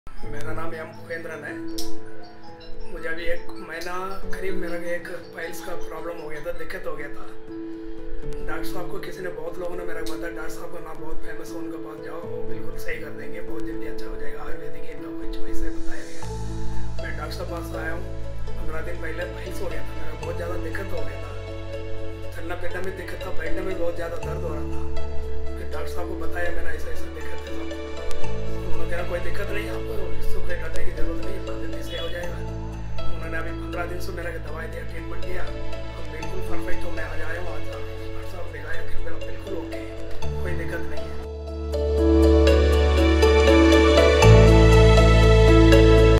Permanent Piles Treatment In Sushruta Anorectal Institute, Delhi
Are you suffering from Piles? Looking for permanent treatment without surgery? We have the solution!!!

We are providing life-changing treatments to all our patients. Watch this patient testimonial Now!!